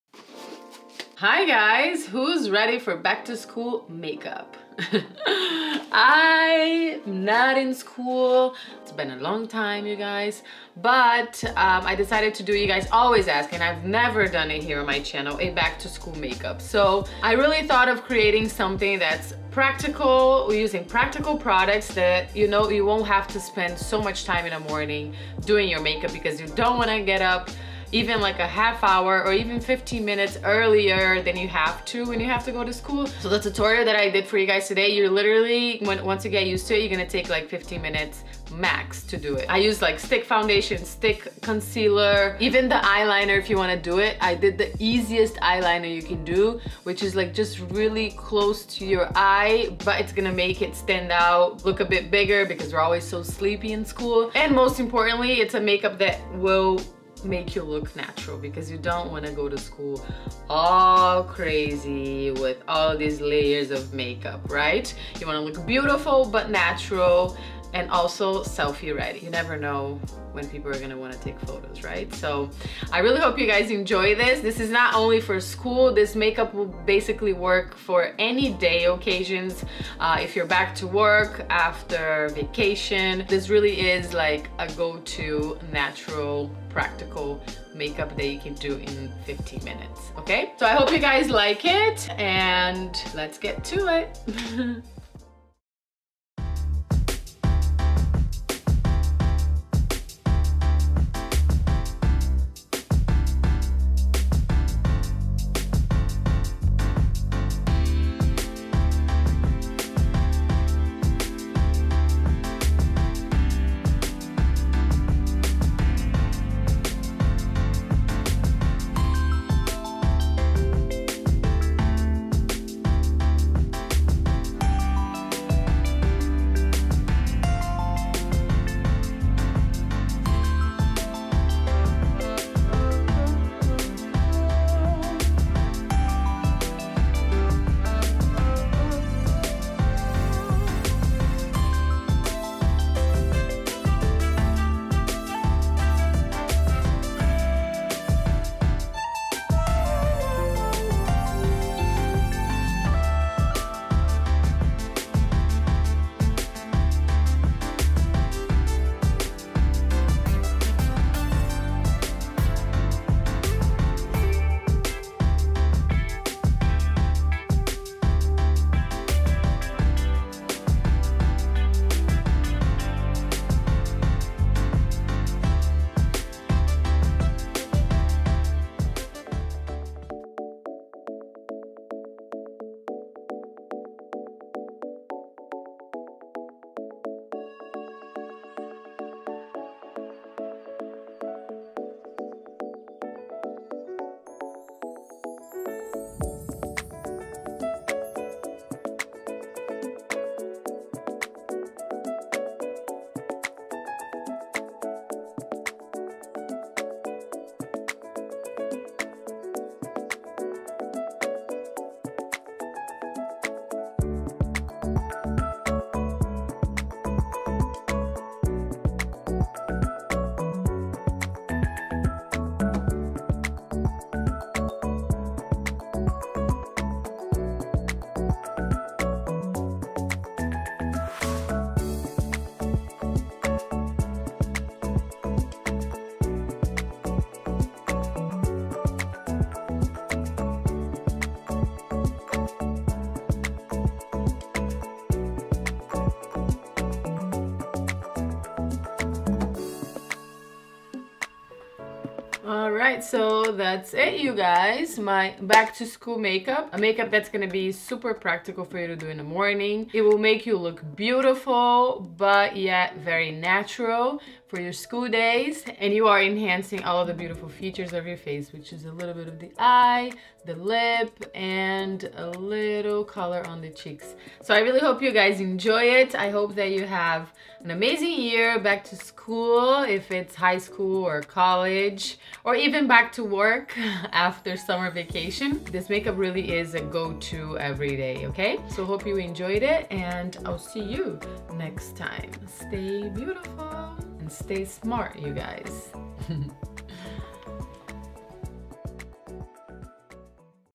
BACK TO SCHOOL MAKEUP!
My loves!

I've never done it before here on my channel, but you guys always ask me to do a back to school makeup so here it is! I created a look that is practical with products that are easy to work with. It's a makeup that is quick, effortless, natural yet still beautiful. I really hope you like it!

I’m On Snapchat: camilacoelho88